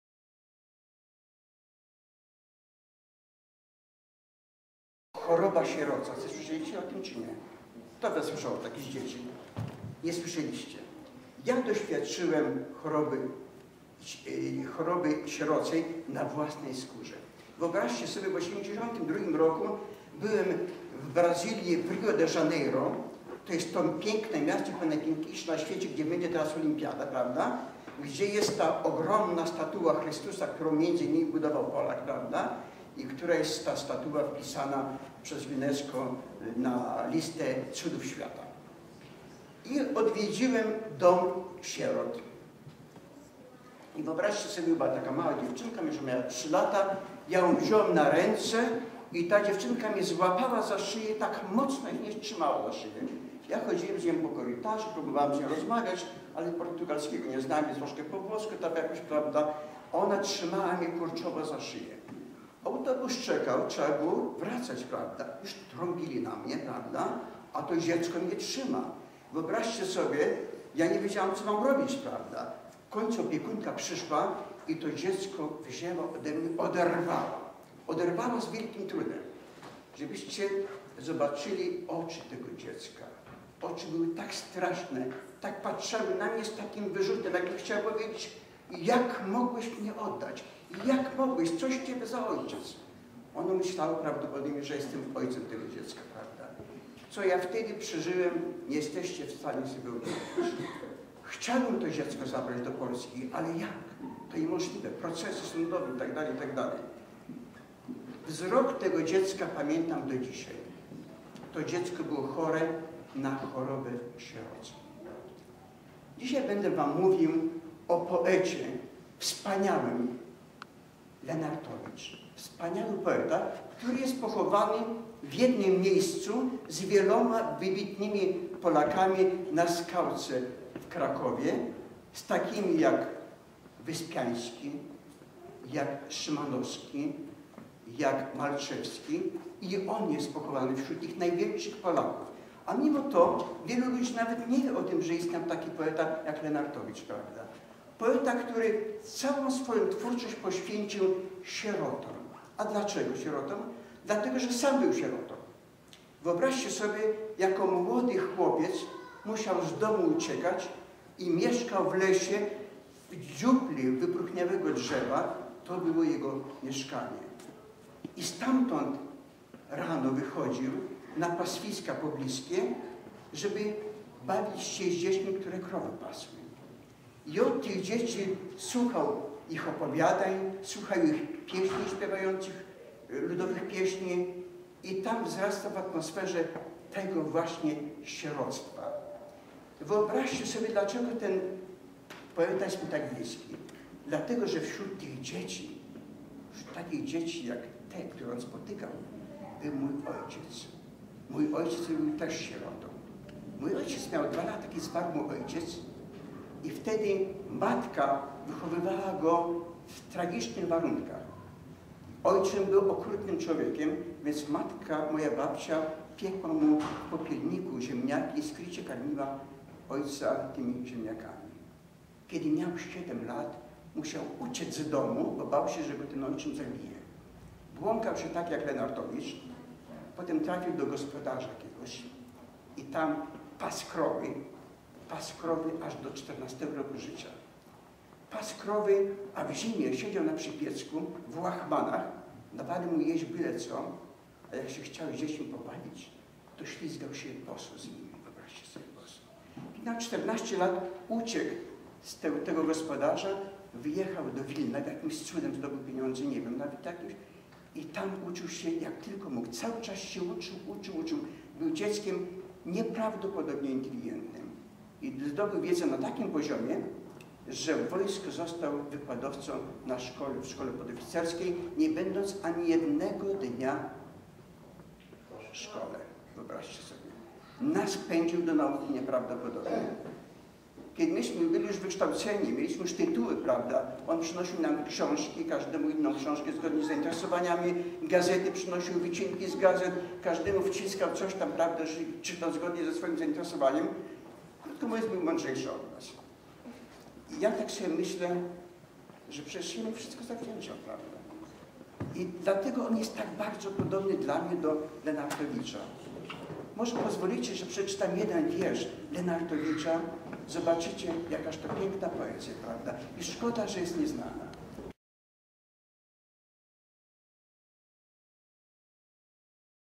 Choroba sieroca
Historyjka dla dzieci opowiedziana przez prof. Jerzego Dobrzyńskiego.